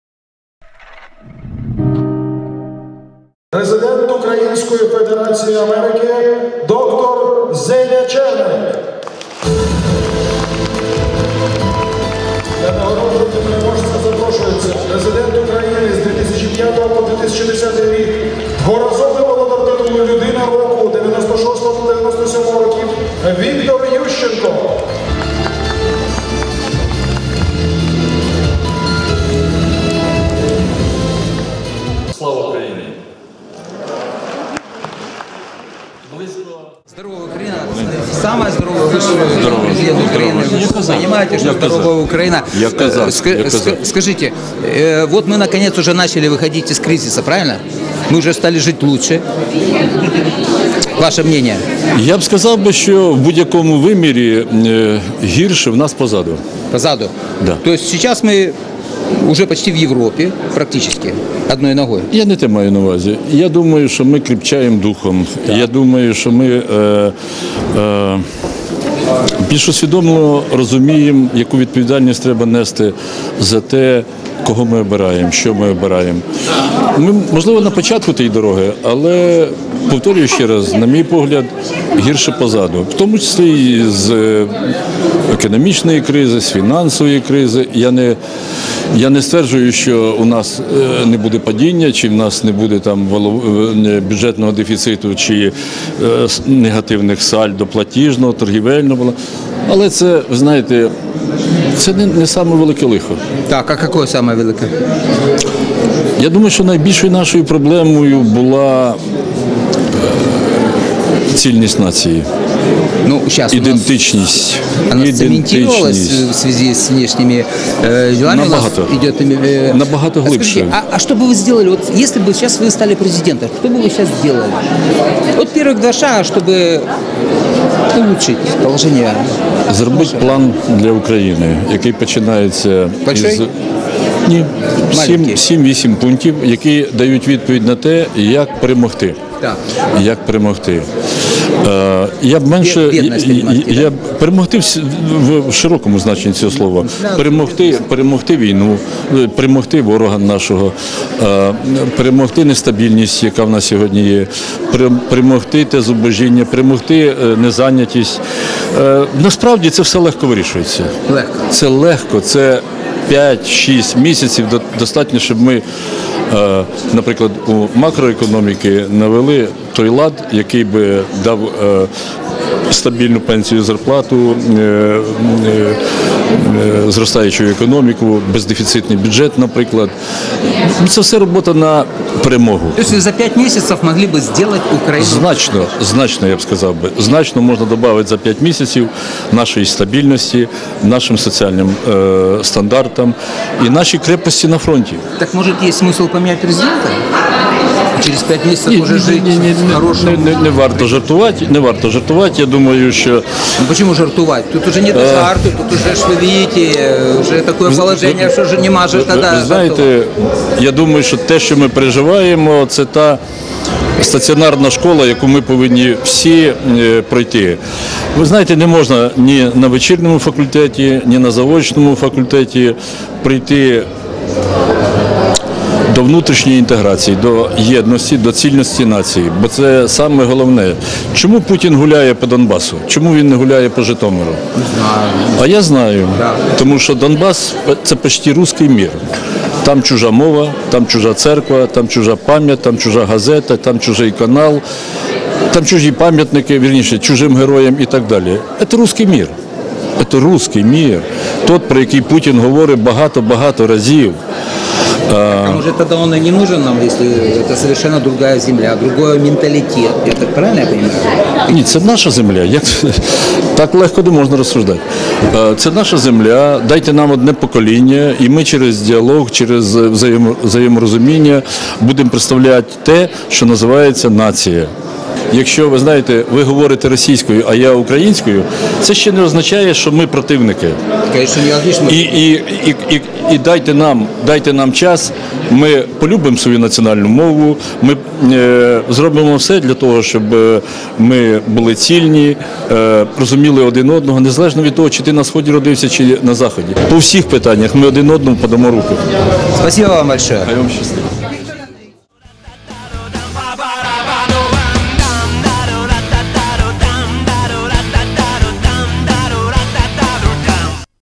Виктор Ющенко - под флагом Америки - Худшие времена позади - Человек года 2016 для Здоровой Украины
Виктор Ющенко (3-й президент Украины) рассказал Липовскому Станиславу (ТВ Здоровая Украина) о том, что худшее в Украине уже позади. Он готов вывести Украину из кризиса за пол года. Донбасс- это русский мир.
20-я юбилейная номинация Человек года - 2016.